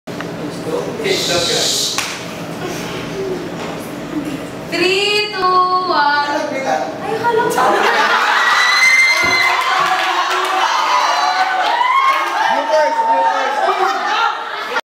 Happy Birthday Miss Talens!!! in 3... 2.... ooops =)))
Akala ko kailangan ko talagang magbilang, bumilang naman ako.
Inaway nila ako hahaha ang lakas ng sampal saken ni Jeremy eh, kung maka-dagok kala mo siya nagpapakain sa'kin araw araw eh :| =))))) HAHAHAHA!

WHY I LOVE YOU GUYS. ♥
Happy Birthday Miss Talens, bukas na omg! :)